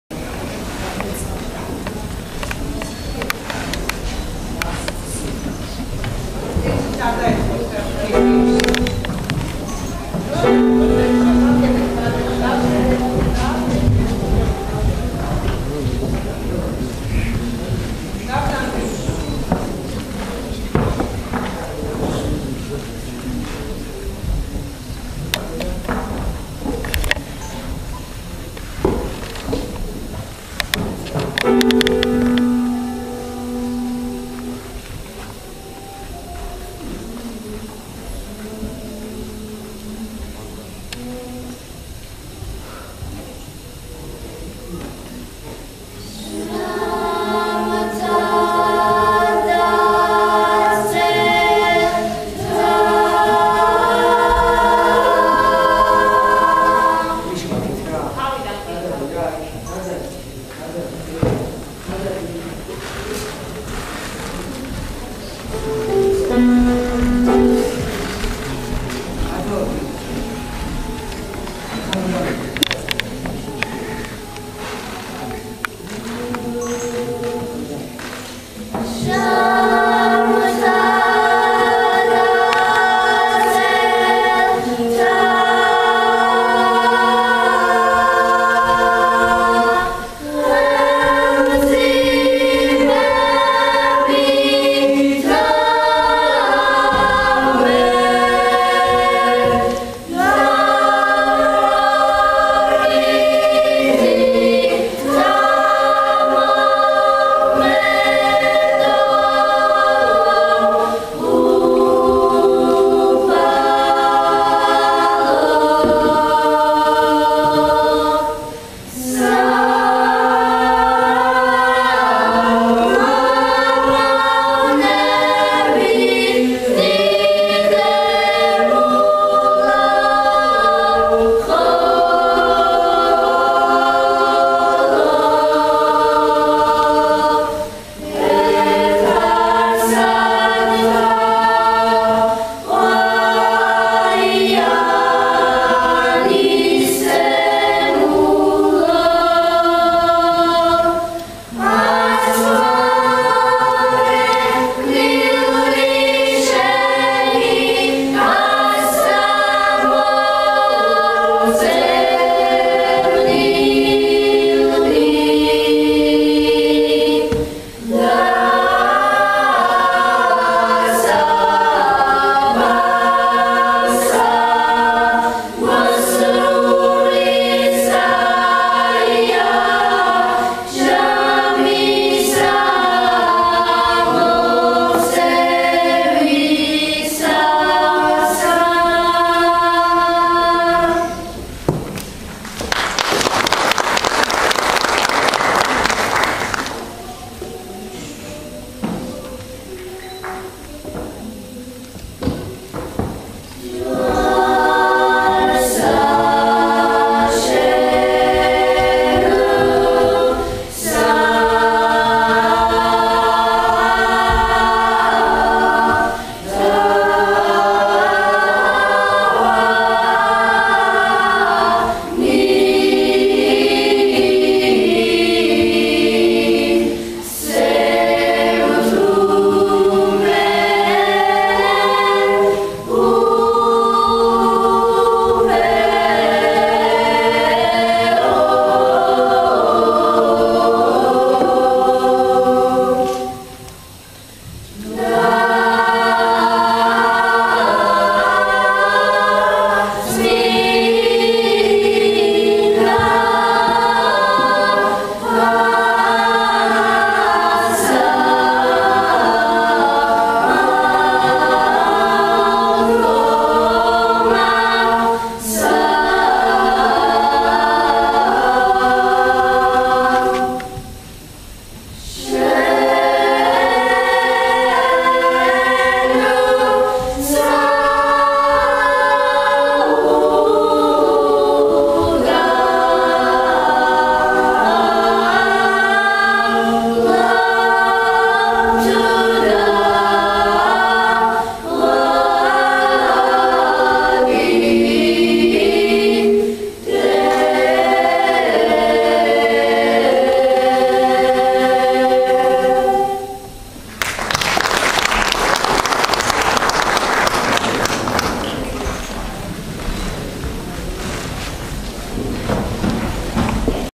საქართველოს ბავშვთა და ახალგაზრდული საგუნდო კოლექტივების II ეროვნული კონკურსის II ტური
საპატრიარქოს ტელევიზია ,,ერთსულოვნება''-ს დარბაზი